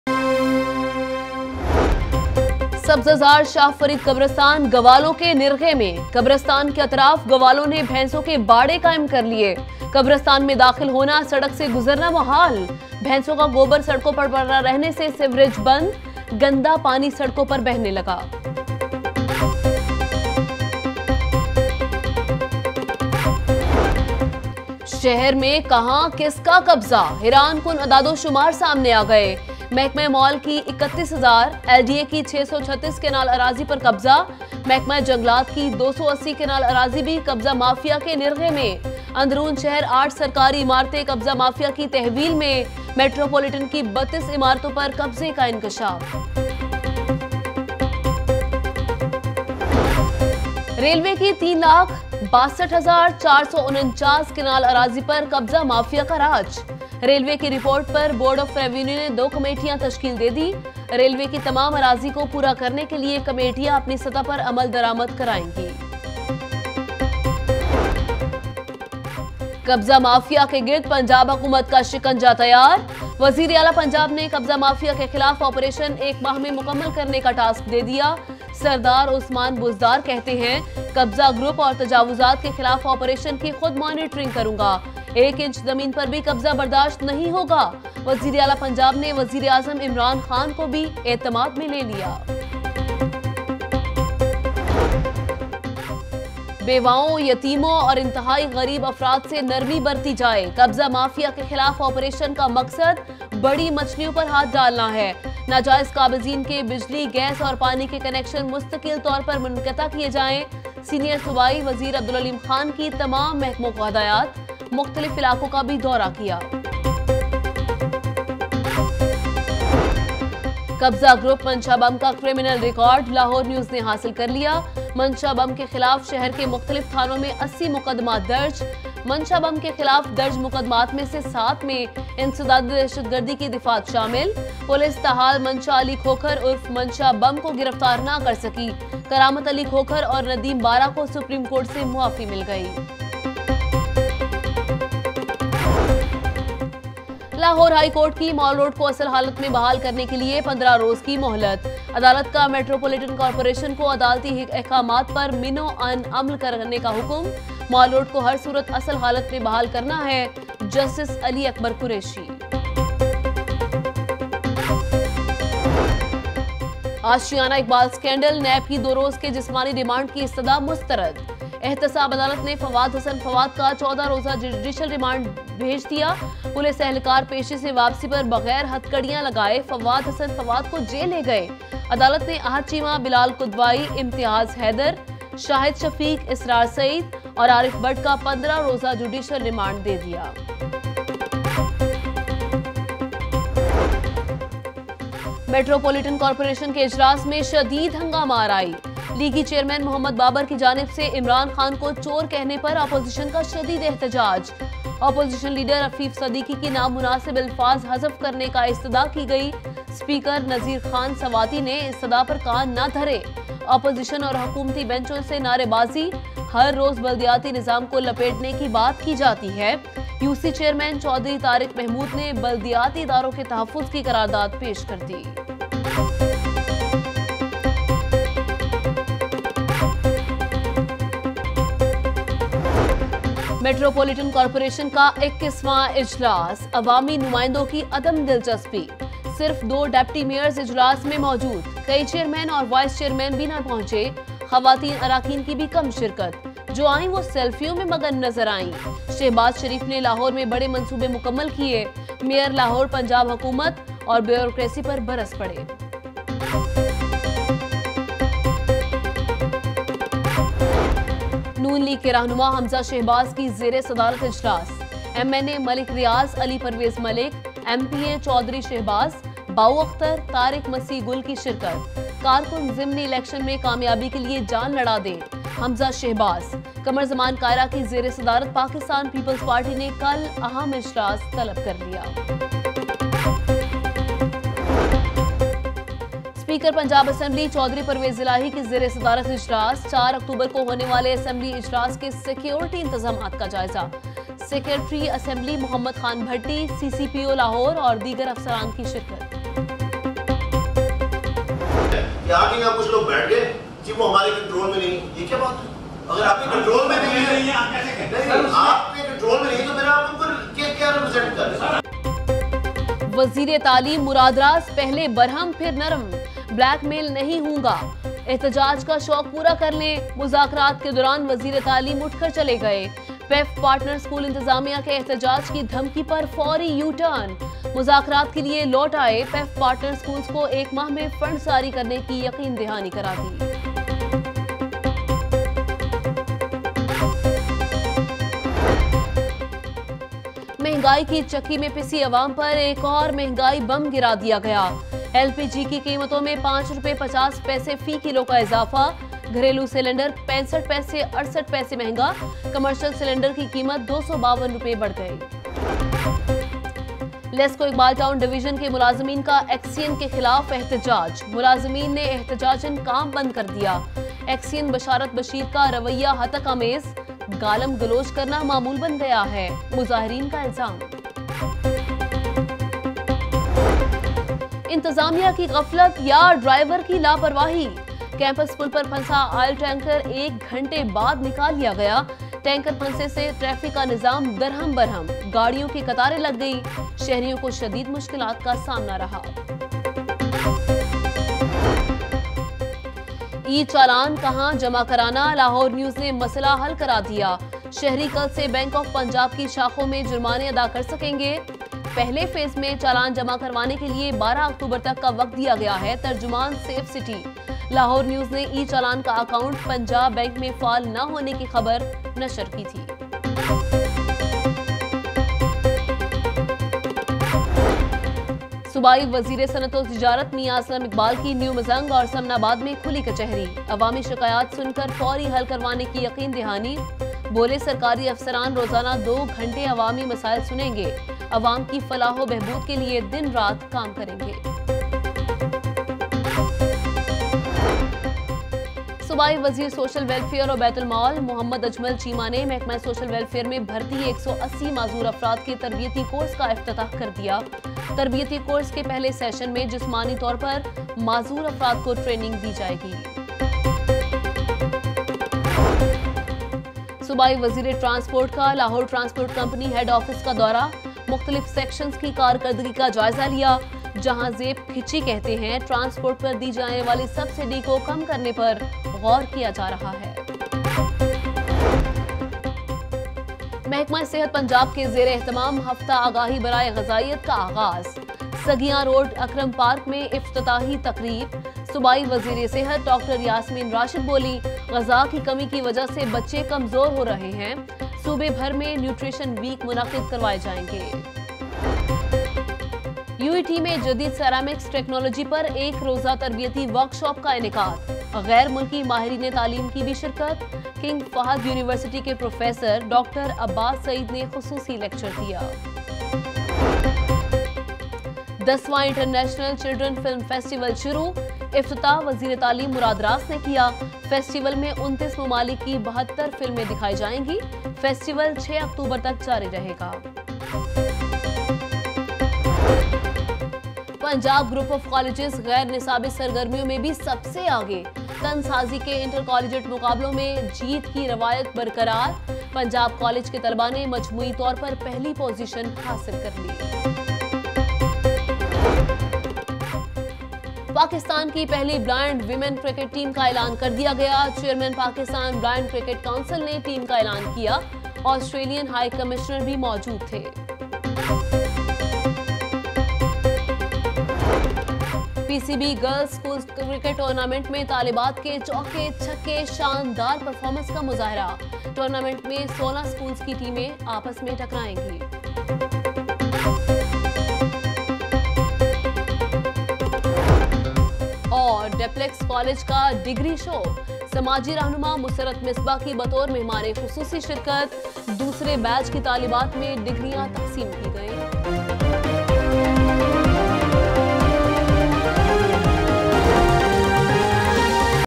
10 PM Headlines Lahore News HD - 01 October 2018
Watch Latest News : Pakistan Railways inaugurates non-stop train from Lahore to Faisalabad, Federal Minister Railways Sheikh Rasheed inaugurates non-stop train from Lahore to Faisalabad, Shahbaz Sharif's son-in-law Ali Imran to appear before court, Court rejects guardianship application of Yousaf Raza Gillani's daughter, Fawad Hassan Fawad to appear before court on 15th October in alleged corruption allegations against him.